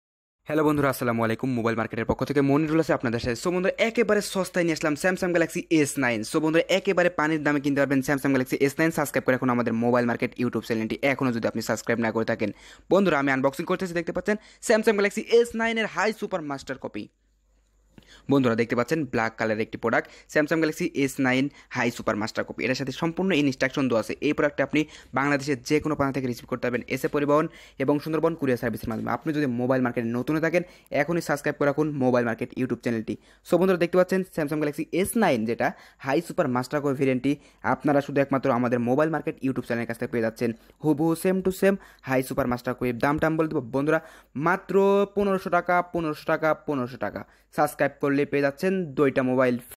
মাত্র 1500 টাকায় কিনুন Samsung Galaxy S9 high super master copy Bangla full review
Samsung Galaxy S9 high super master copy.